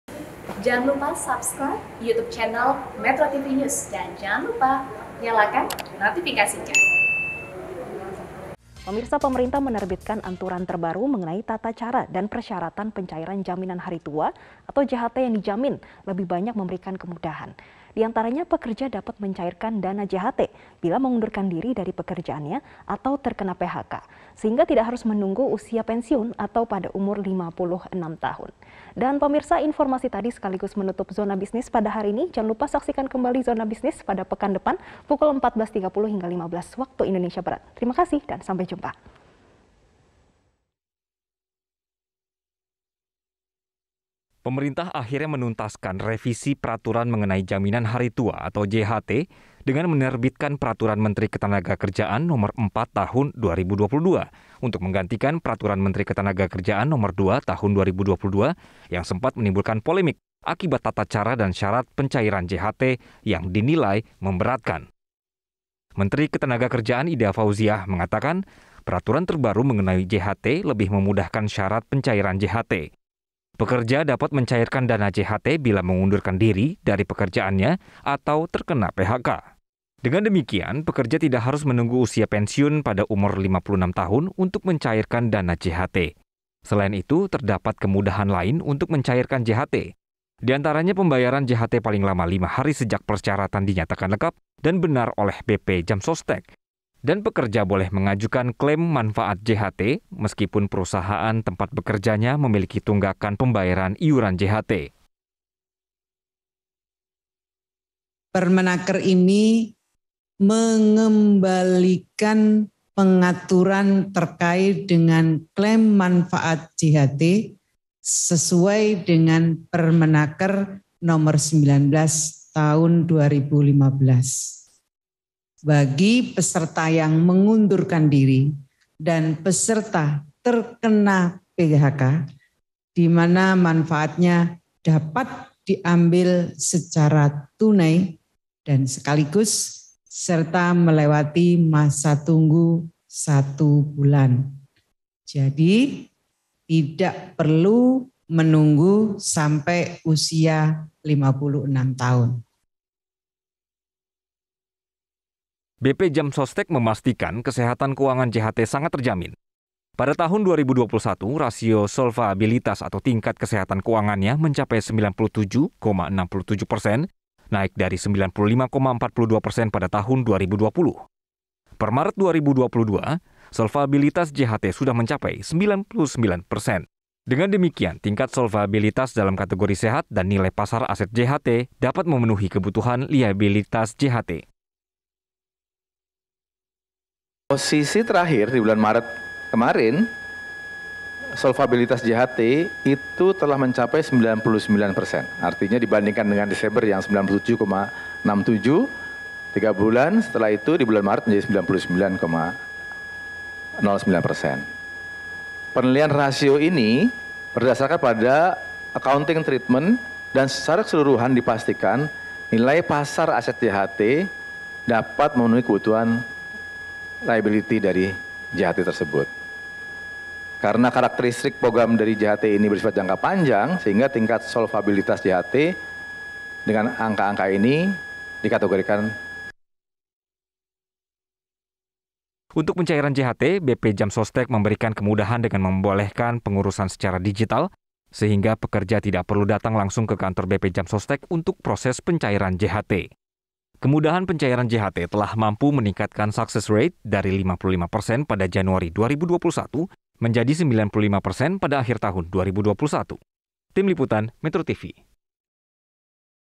Aturan Diubah, Pencairan Dana JHT Lebih Mudah
Pemerintah menerbitkan aturan baru mengenai tata cara pencairan dana Jaminan Hari Tua (JHT) yang memudahkan masyarakat. Pekerja dapat mencairkan dana JHT bila mengundurkan diri dari pekerjaan atau terkena PHK tanpa harus menunggu usia 52 tahun.